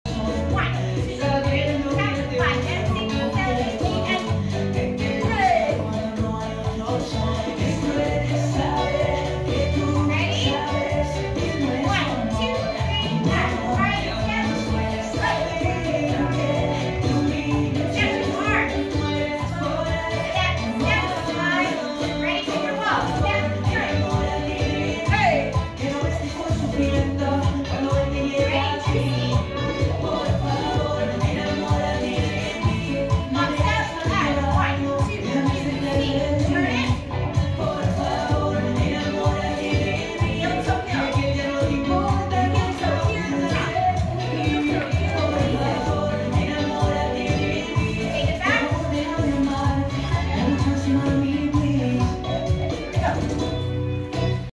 Bachata Footwork Open Level at Free Spirit
In this class you will learn bachata footwork mixing Dominican Bachata (where bachata originated) and a side dash of Bachata Sensual. A little mix of everything to give your footwork flavor on the dance floor!

Open level classes are for experienced beginner to intermediate. This is not a partner-work class.